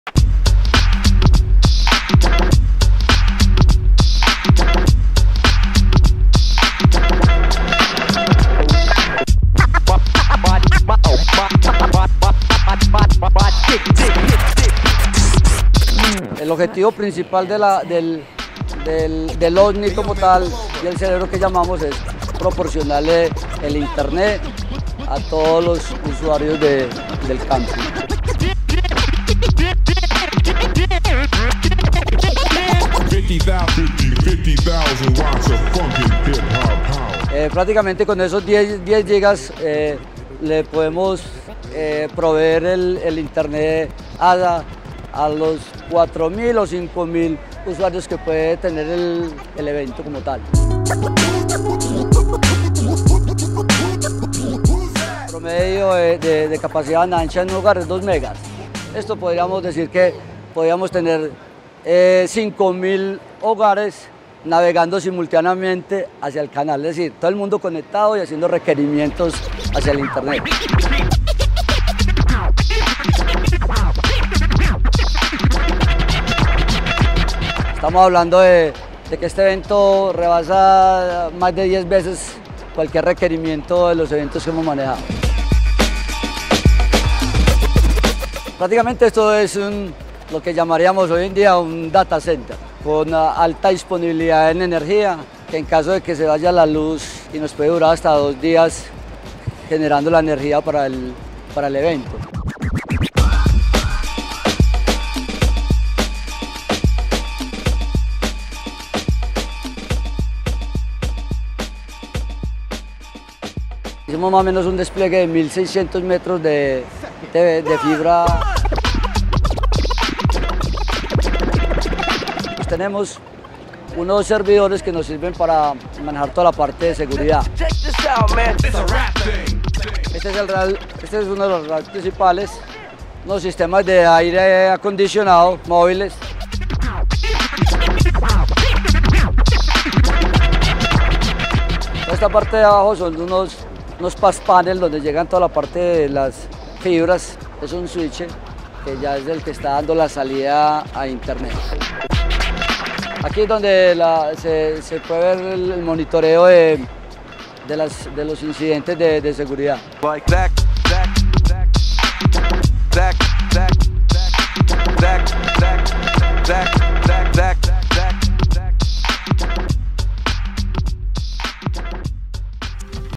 Cerebro Campus Party 2013
Campus Party se mueve por un centro de internet, que algunos llaman Cerebro y que según los creadores del evento, ellos, lo denominaron el Ovni.

Este cerebro está dispuesto para proveer de internet a todos los visitantes del Campus Party y nosotros, estuvimos dentro de él, caminando con cuidado para no ir a desconectar los cables y conociendo cómo era su montaje. Veamos.